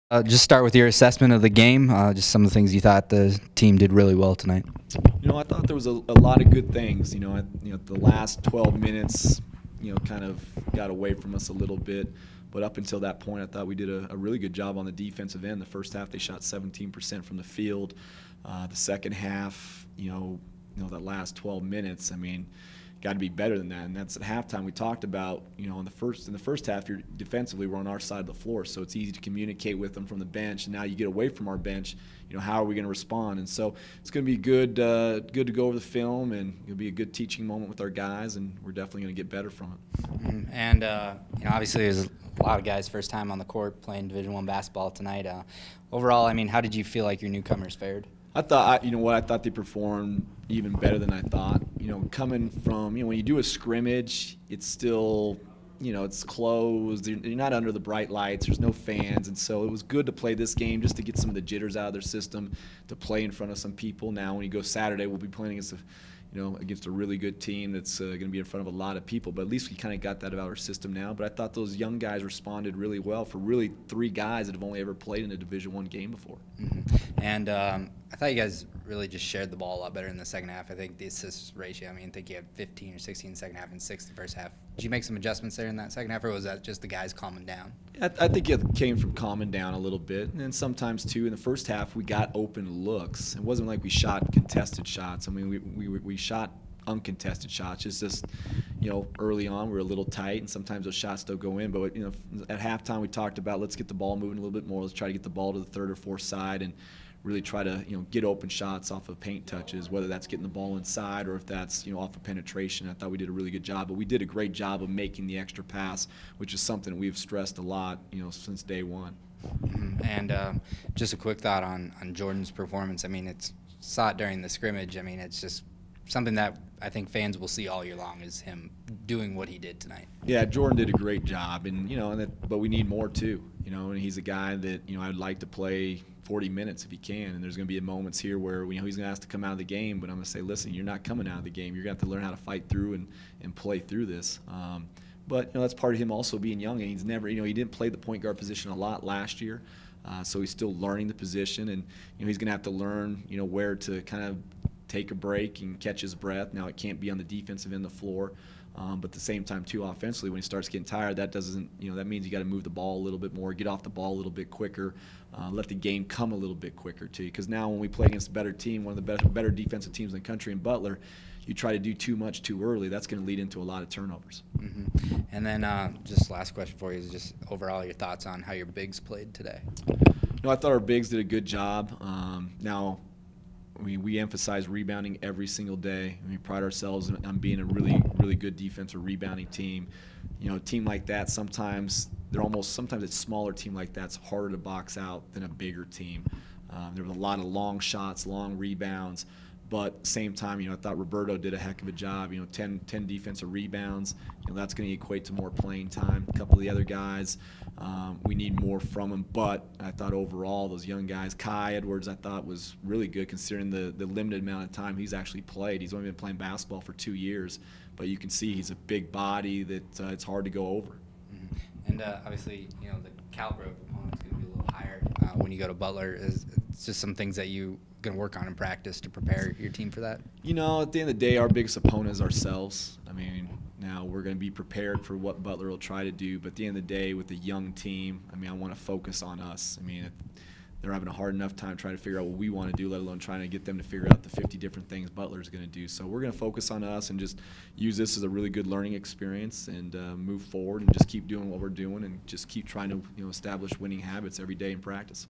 Coach's Press Conference: JWU
Coach Jeff Linder talks about how the team is improving and adapting to better teams as well as bigger crowds.